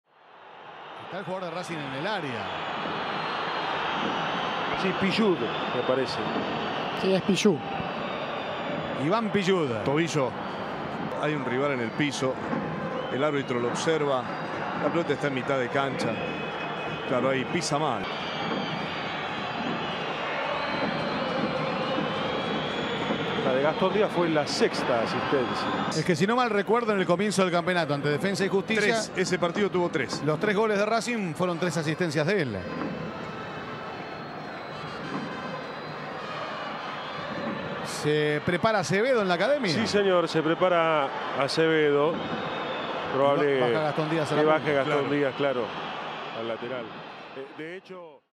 Pisó mal y se tuvo que ir de la cancha.Estudiantes-Racing.Fecha 11.Torneo Primera División 2014.FTP
Iván Pillud salió lesionado y le dejó su lugar a Acevedo. El lateral mostró una molestia en el tobillo y tuvo que retirarse.